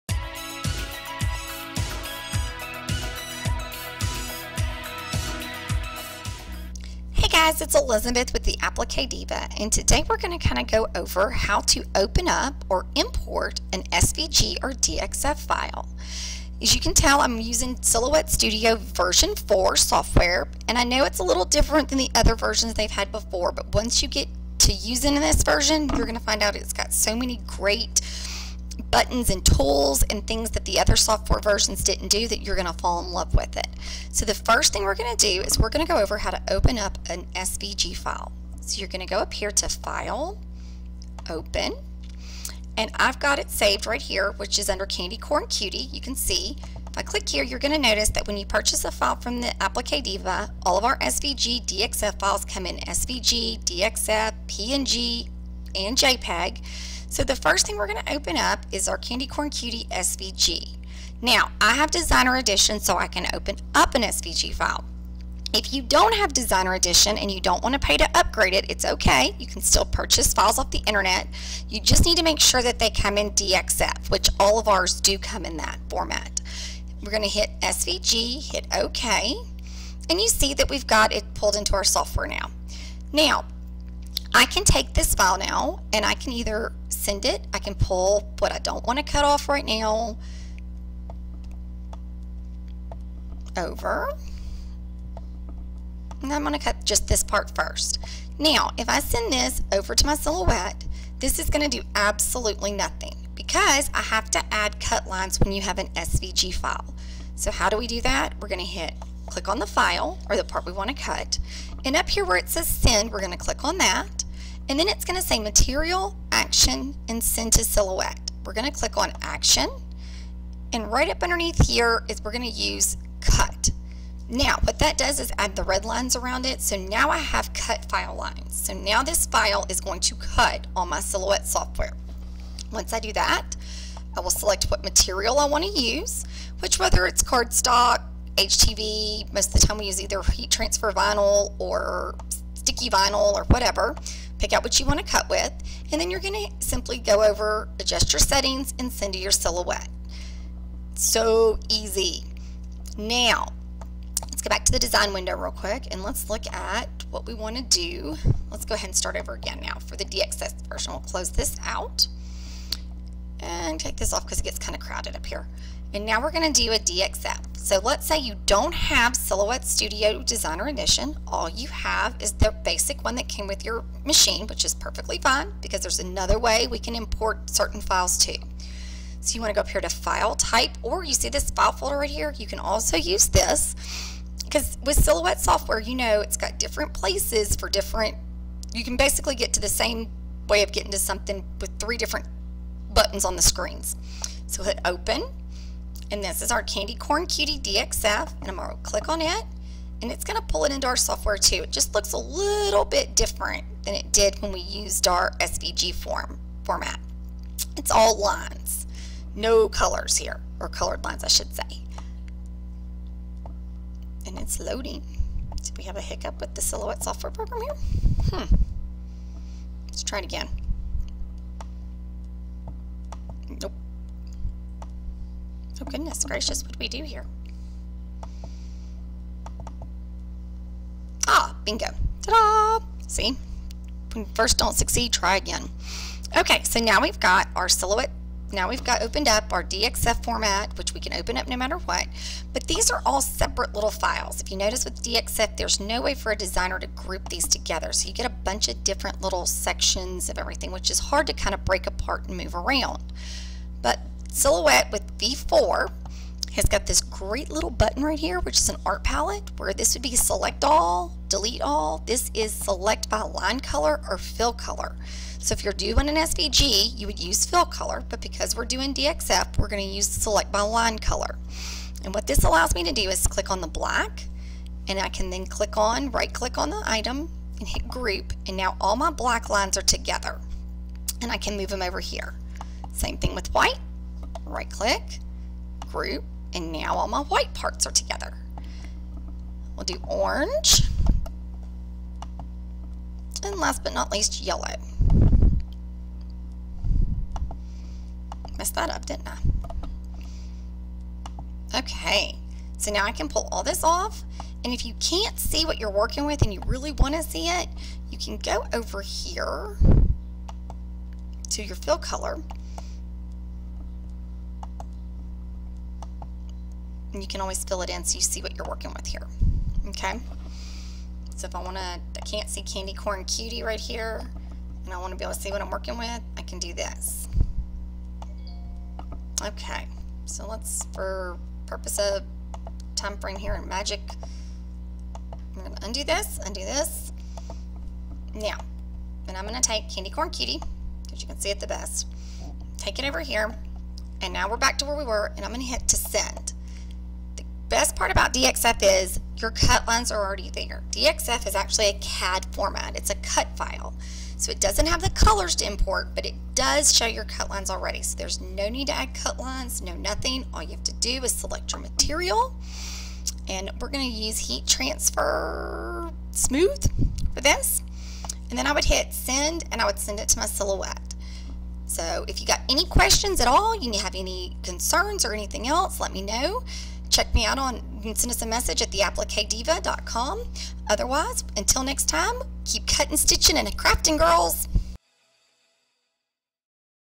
How to import and open SVG and DXF files in Silhouette Sutdio
This is a quick video on the basic ways to open a SVG or DXF file with the Silhouette Studio Software.